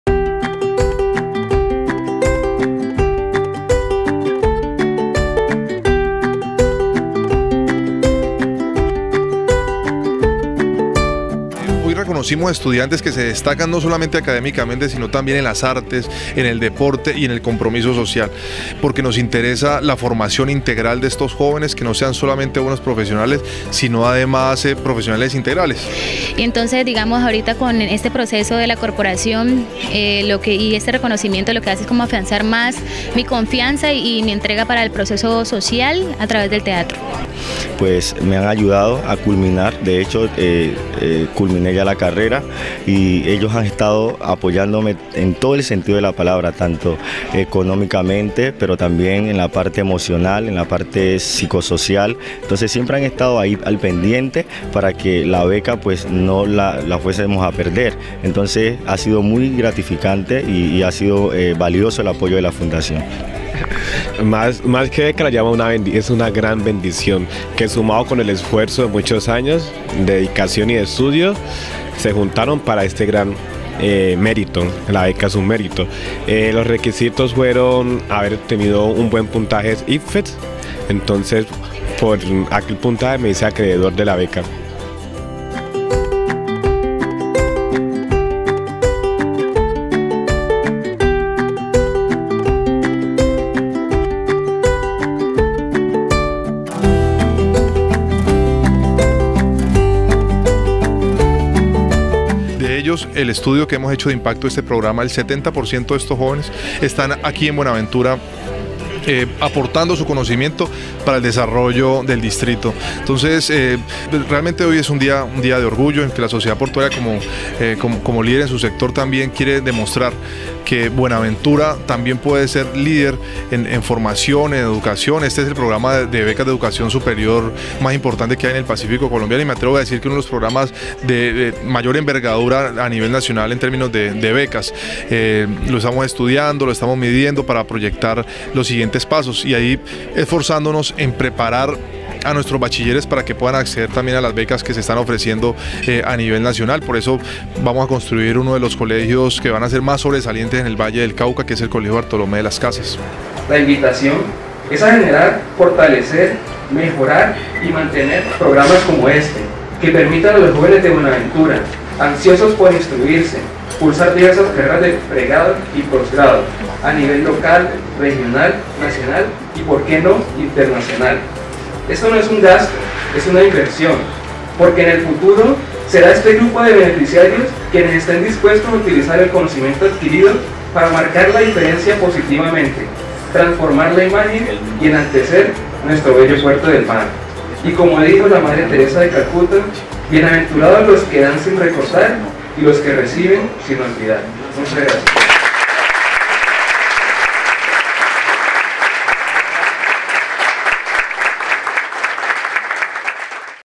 EXALTACIÓN PILOS 2016
Fundación Sociedad Portuaria Buenaventura, FSPB, en el marco de Pilos 2016, reconocimiento anual con el objetivo de motivar a los estudiantes a superar sus metas académicas, premió de manera integral a becados de educación superior, estudiantes del Colegio Bartolomé de las Casas, Maestro Destacado y Pilos MEN apoyados por la FSPB y FundaciónColordeColombia. @DavidStivenArboledaPrado @LuisAntonio AlegríaSalazar @AngieYulierCardonaSuárez